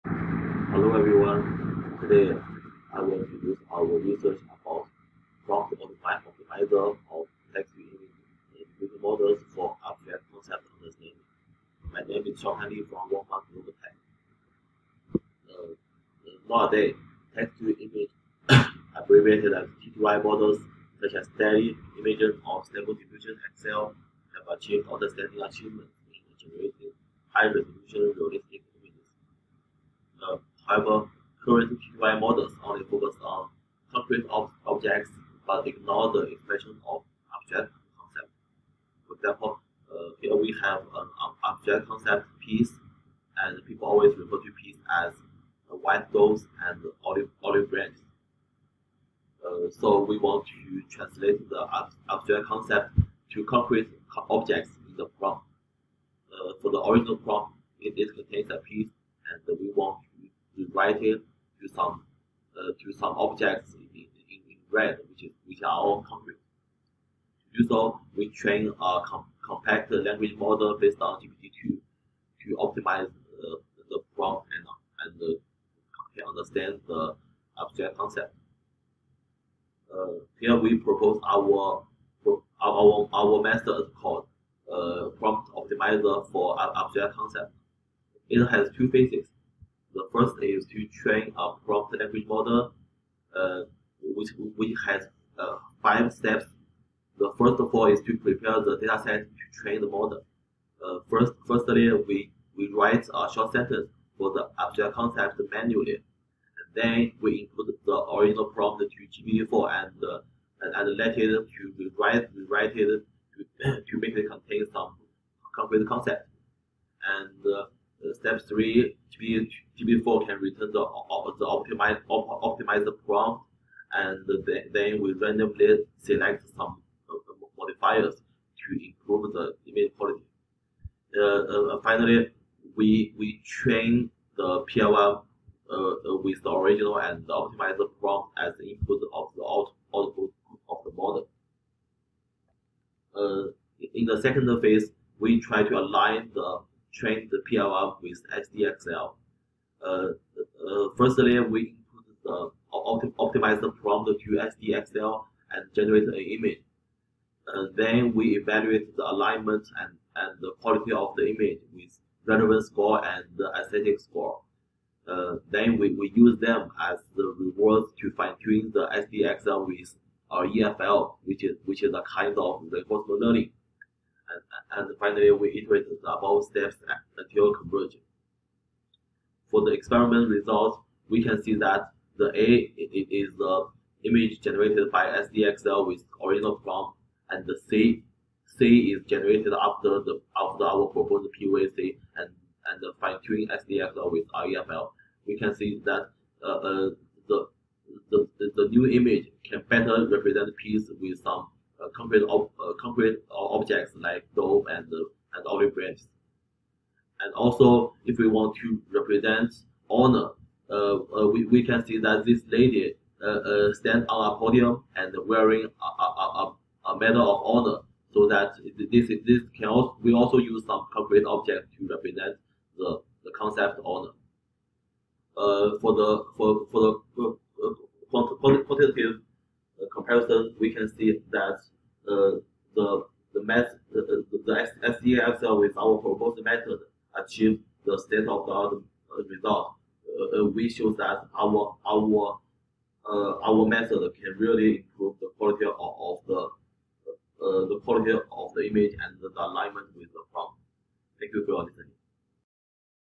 [w06d0111] Prompt Optimizer of Text-to-Image Diffusion Models for Abstract Concept Understanding
"Prompt Optimizer of Text-to-Image Diffusion Models for Abstract Concept Understanding

Zezhong Fan, Xiaohan Li, Kaushiki Nag, Chenhao Fang, Topojoy Biswas, Jianpeng Xu and Kannan Achan"